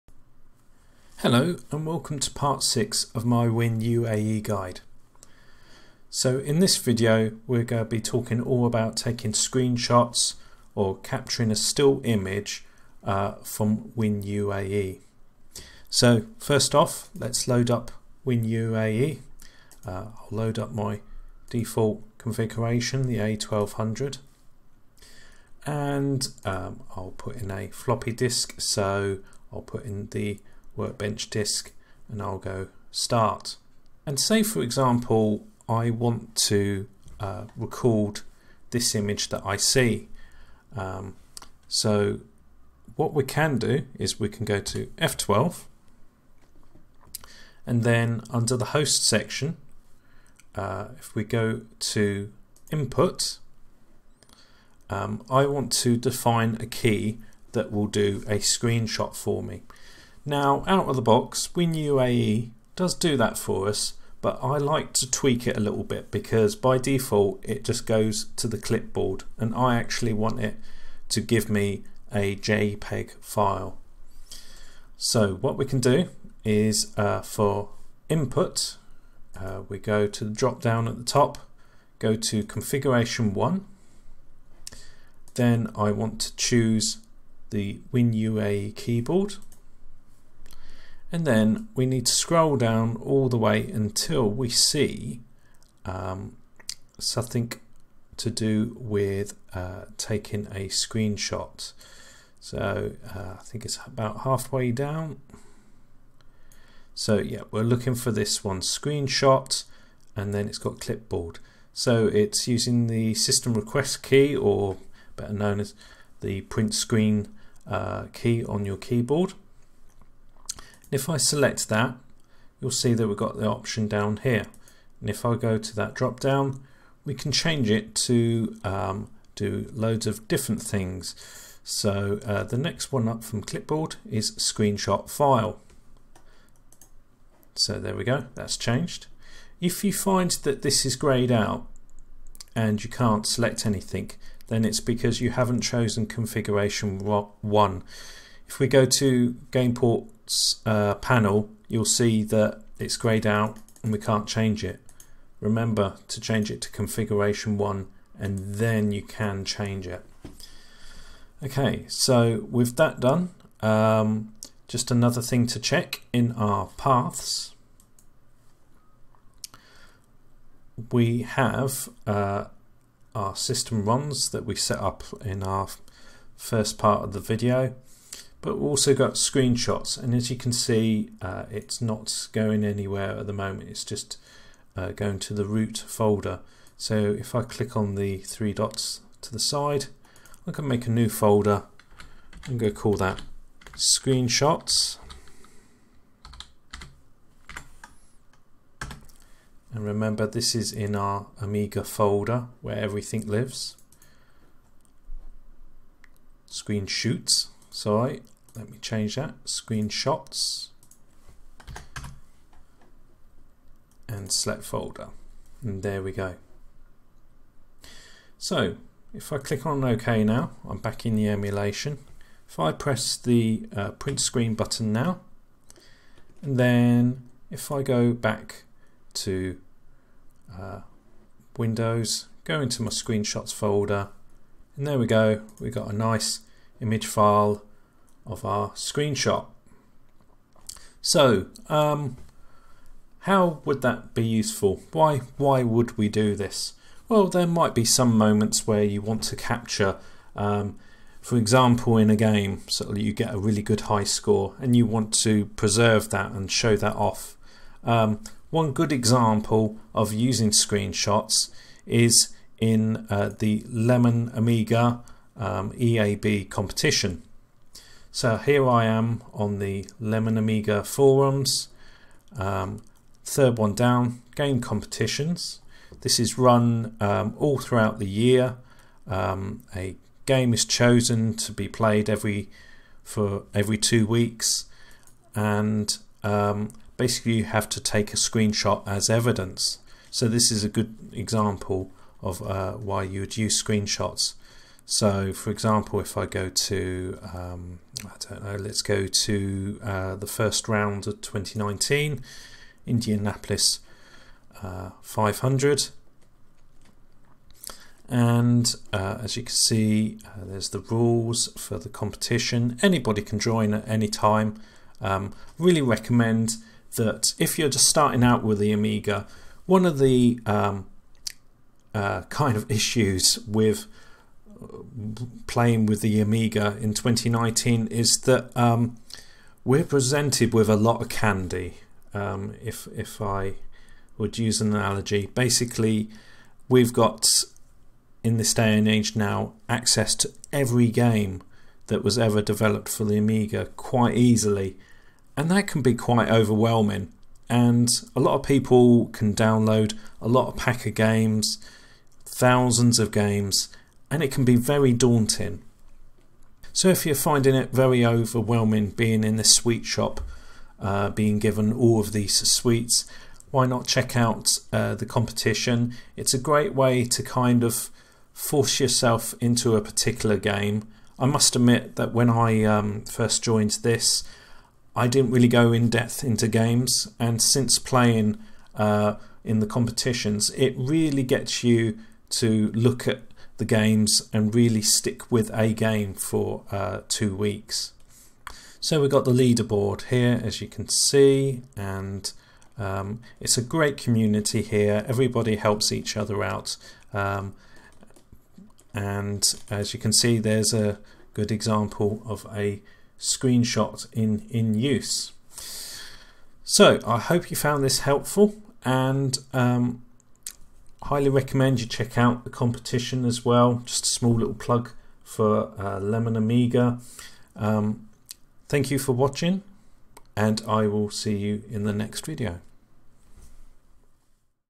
WinUAE Guide - Part 6 - Screenshots
In this next part of looking at WinUAE. We explore how to take screenshots of our Amiga.
And I also show a bit of the LA / EAB Competition.